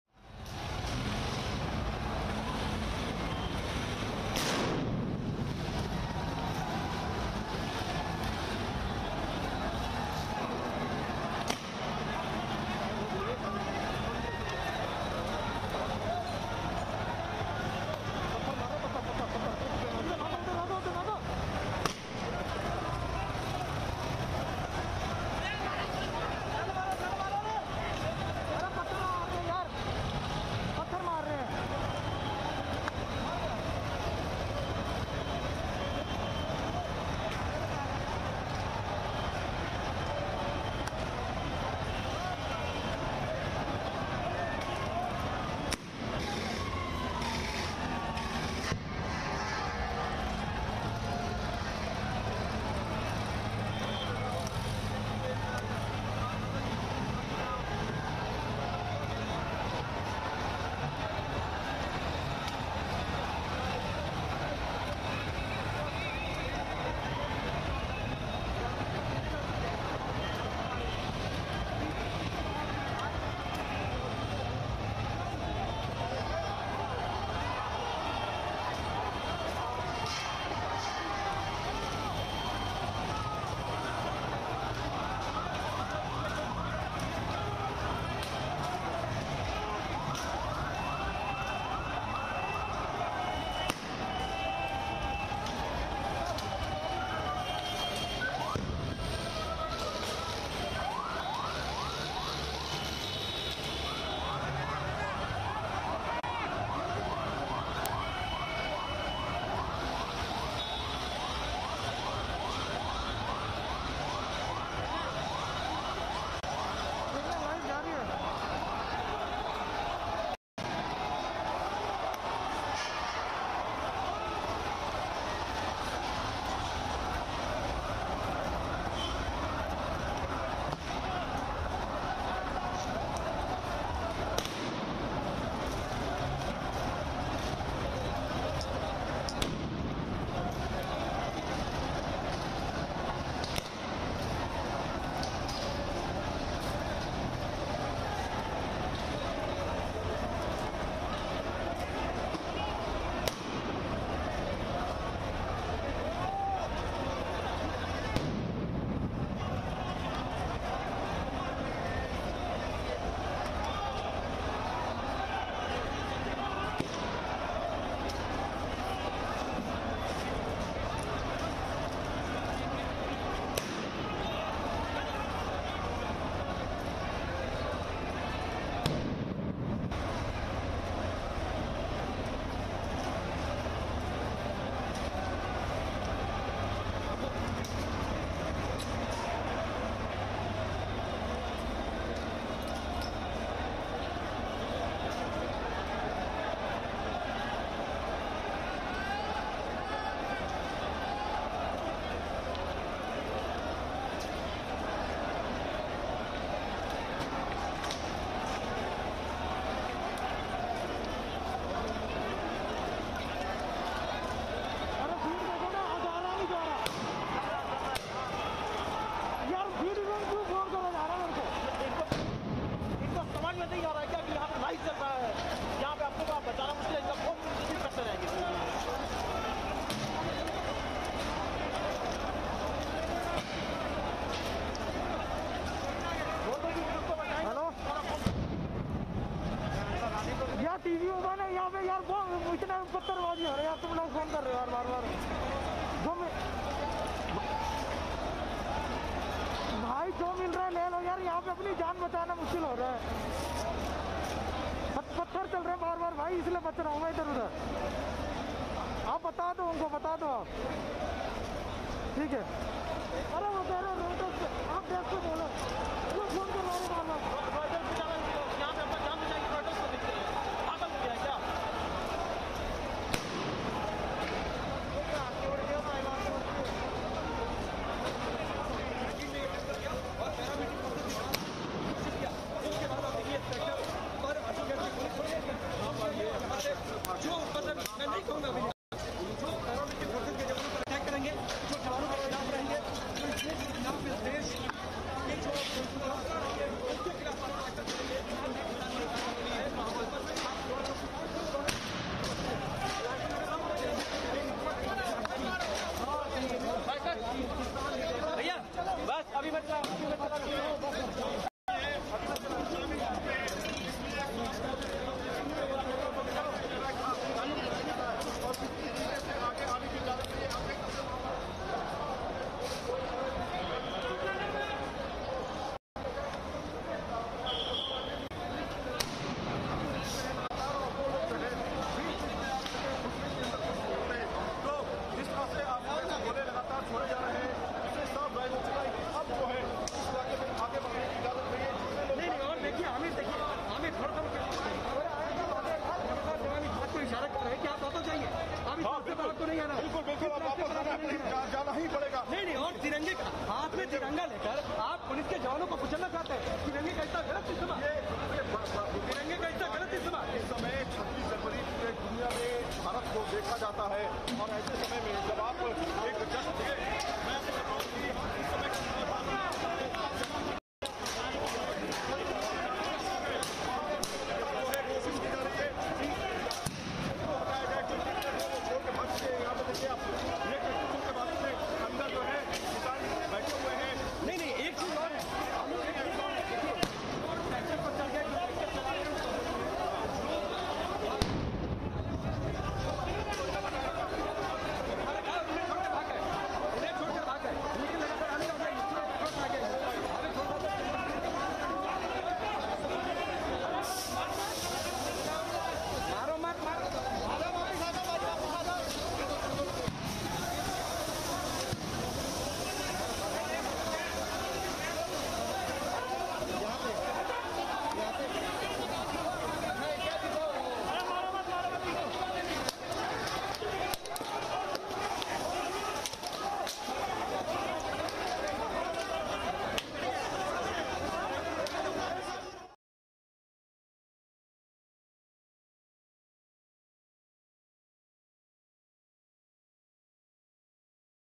Farmer Protest In Nangloi: Police fired tear gas shells after a clash with farmers
Farmers’ tractor rally Live Updates: Indian farmers protesting against agricultural reforms breached barricades and clashed on Tuesday with police in the capital, who fired tear gas to restrain them, shortly after a convoy of tractors trundled through the city's outskirts.

Hundreds of protesters, some on horseback, broke away from the main route of the tractor protest to head for central New Delhi, which hosts an annual Republic Day Parade.

They commandeered cranes and used ropes to tear down roadblocks miles from routes approved by the police, forcing constables in riot gear to fall back and let them pass.
The Delhi Police has said the protesting farmers violated conditions agreed for the tractor rally and resorted to violence and vandalism. "Farmers began tractor parade before the scheduled time, resorted to violence and vandalism.

"Farmers' protest led to extensive damage to public property, many policemen injured," the police said in a statement, accessed by news agency PTI.

Watch: Farmer Protest In Nangloi: Police fired tear gas shells after a clash with farmers